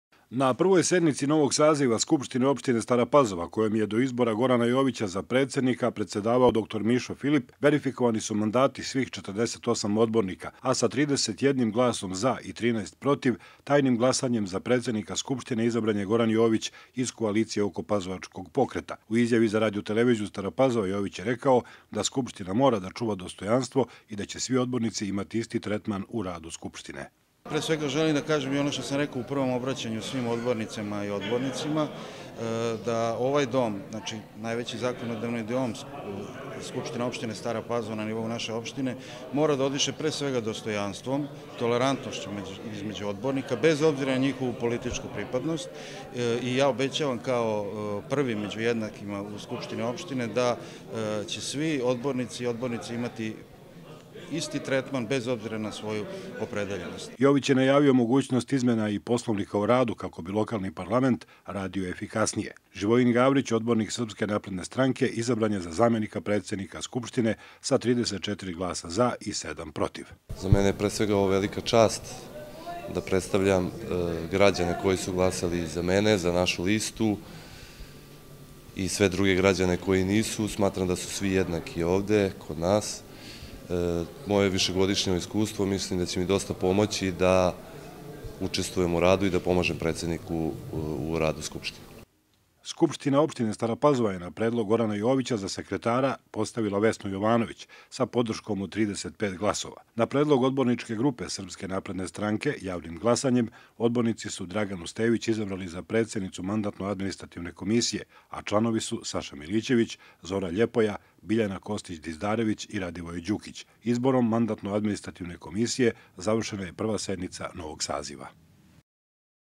Prva SO Stara Pazova
Na konstitutivnoj sednici Goran Jović izabran je za predsednika Skupštine opštine Stara Pazova, a za zamenika je imenovan Živojin Gavrić.